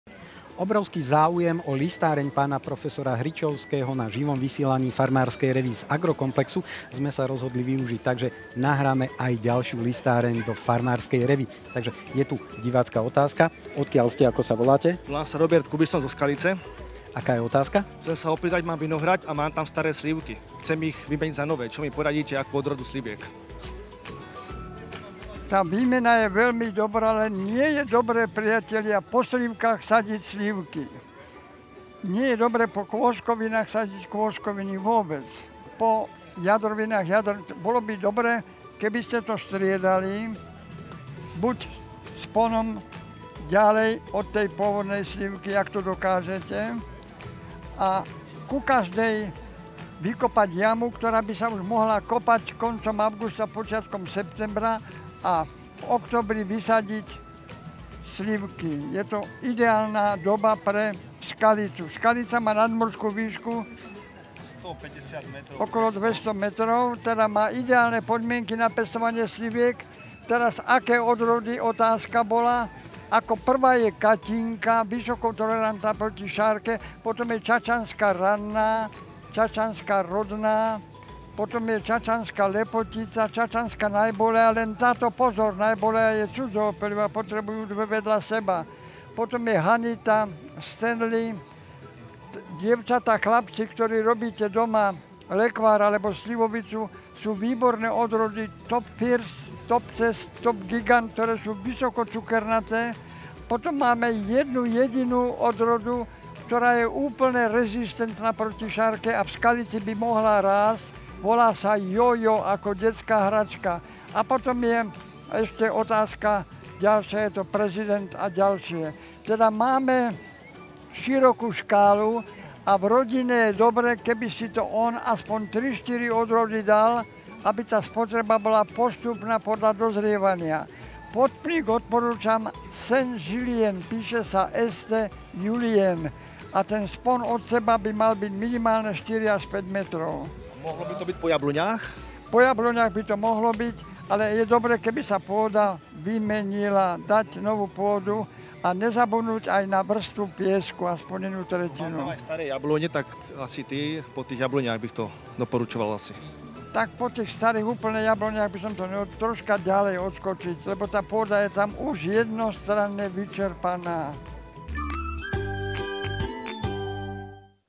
Listáreň na Agrokomplexe
Počas AK sme nakrútili aj toto vydanie listárne s prof.Hričovským. Pozrite si ju!